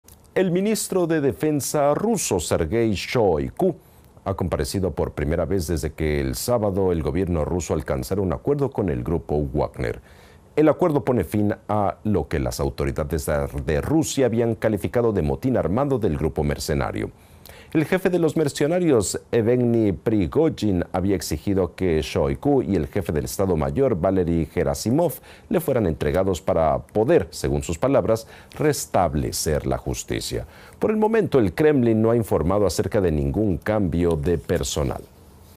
El ministro de Defensa ruso comparece por primera vez desde el motín del Grupo Wagner
El ministro de Defensa ruso, Sergei Shoigu, ha comparecido por primera vez desde que el sábado el Gobierno ruso alcanzara un acuerdo con el Grupo Wagner. El acuerdo pone fin a lo que las autoridades de Rusia habían calificado de motín armado del grupo mercenario. El jefe de los mercenarios, Evgeny Prigozhin, había exigido que Shoigu y el jefe del Estado Mayor, Valeri Gerasimov, le fueran entregados para poder, según sus palabras, restablecer la justicia. Por el momento, el Kremlin no ha informado acerca de ningún cambio de personal.